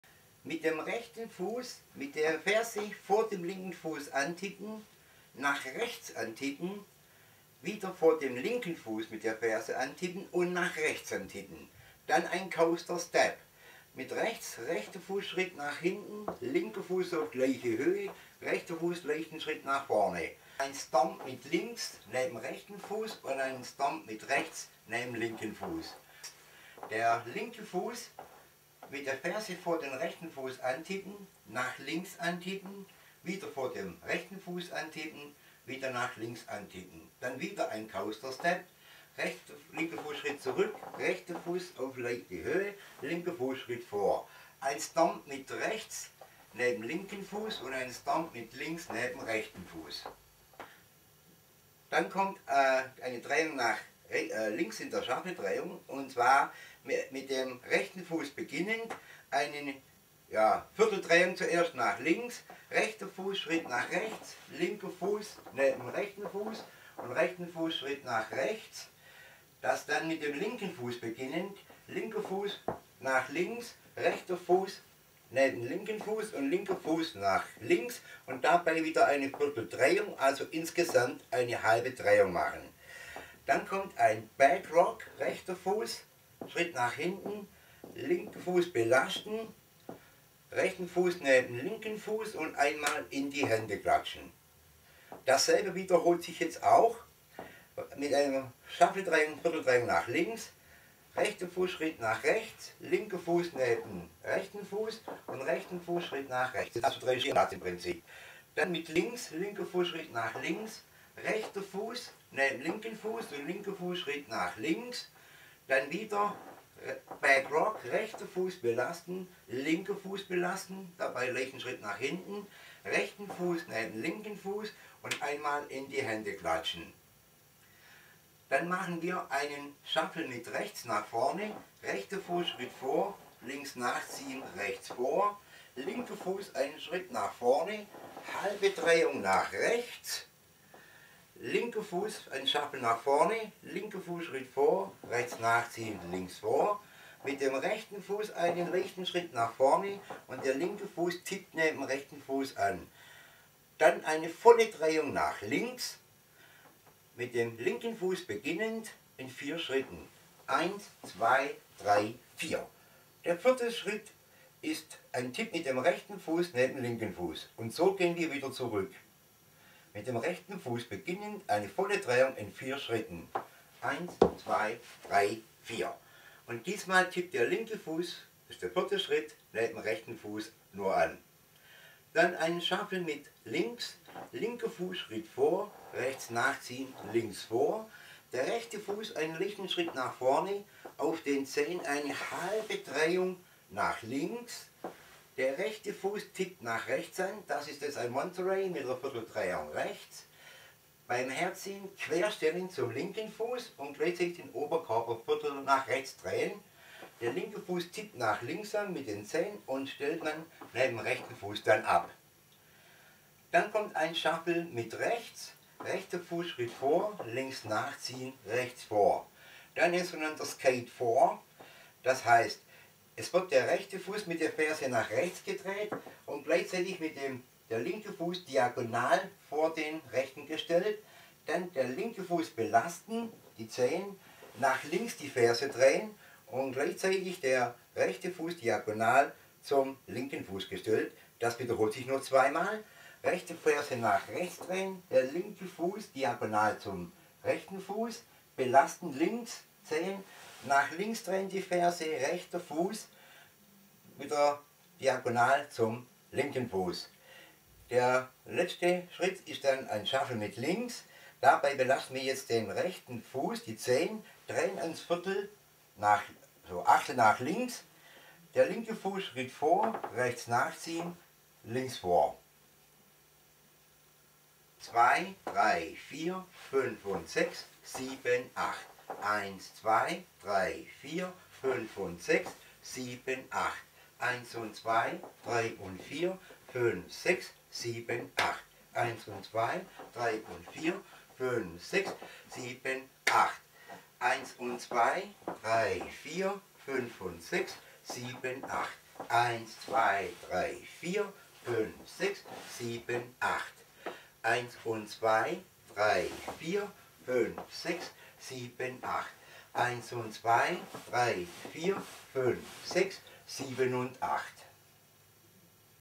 Stargate One - Line Dance - 2013 - Teach
Stargate One - Line Dance - 2013 - Intermediate - 4 Wall - 64 Count -
Choreographie: Walter (The Dancer) American Rebels
Schritt für Schritt Erklärung in Deutsch